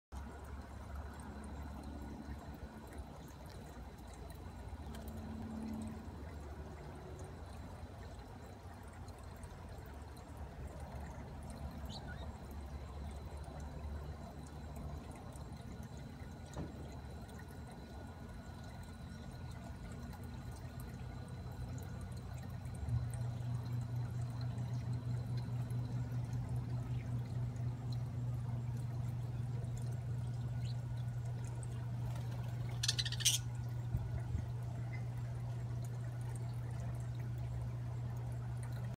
Parakeets Digging in the Dirt
My budgerigars like to explore the flower pots and dig around.